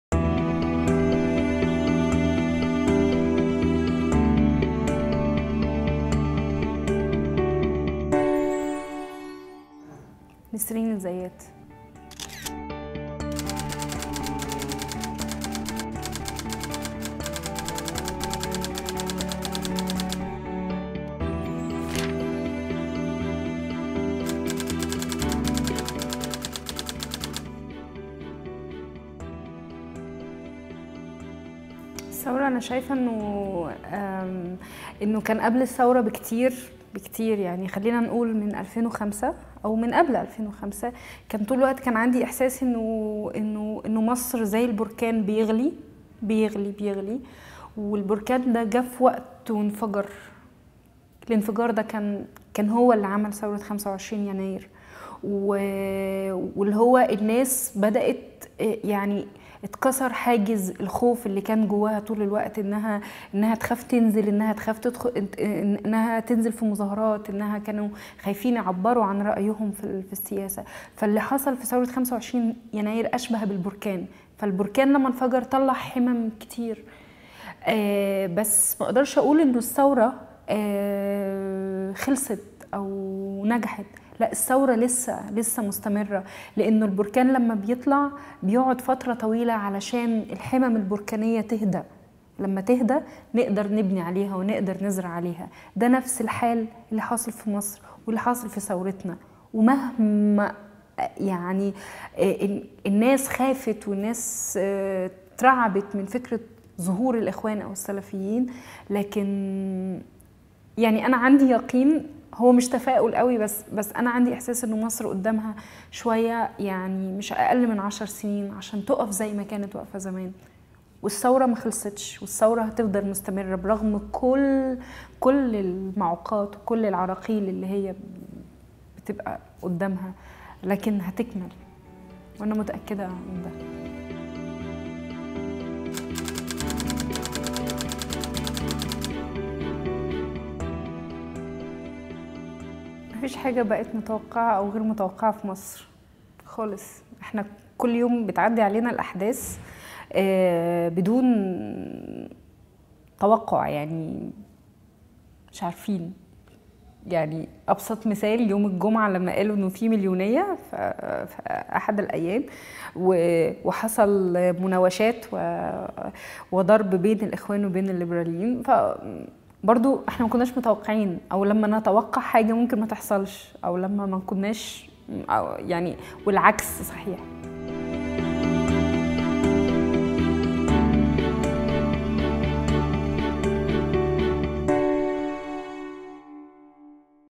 Nesrin El Zayat, Filmmaker, Egypt
"Seit 2005 kochte Ägypten wie ein Vulkan."
"Since 2005 Egypt was boiling like a volcano."